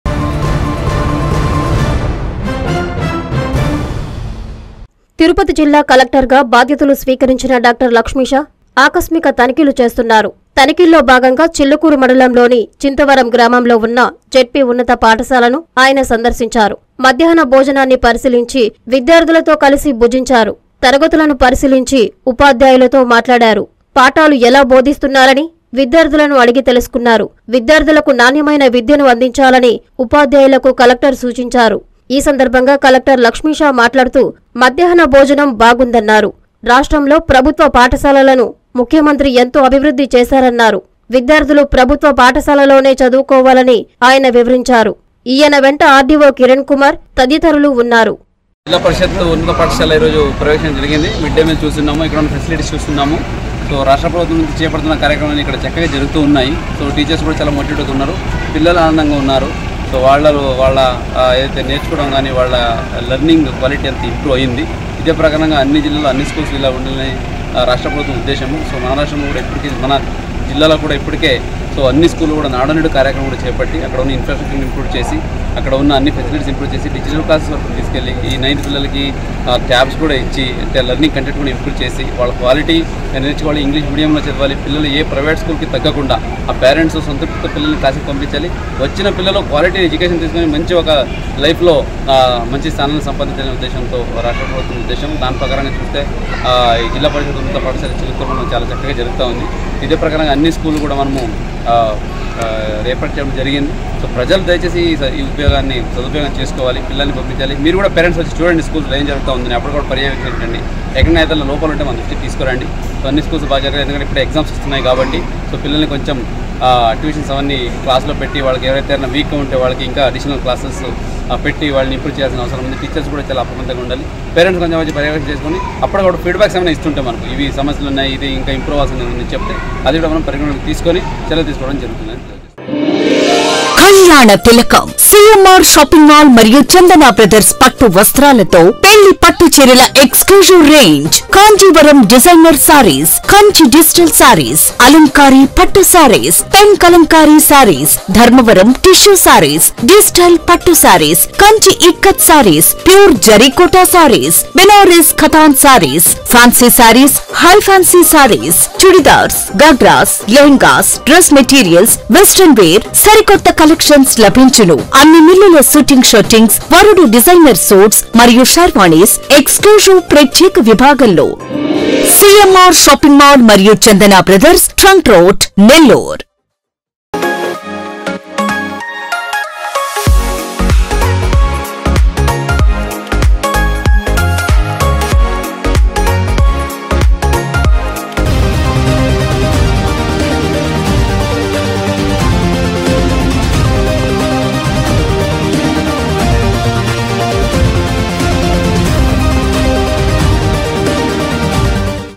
భోజ‌నం బాగుంది
- విద్యార్థుల‌కి నాణ్య‌మైన విద్య‌నందించాలి
- తిరుప‌తి జిల్లా నూత‌న క‌లెక్ట‌ర్ డాక్ట‌ర్ ల‌క్ష్మీ షా
- చింత‌వ‌రంలోని జ‌డ్పీ ఉన్న‌త పాఠ‌శాల‌ను ఆక‌స్మికంగా త‌నిఖీ చేసిన క‌లెక్ట‌ర్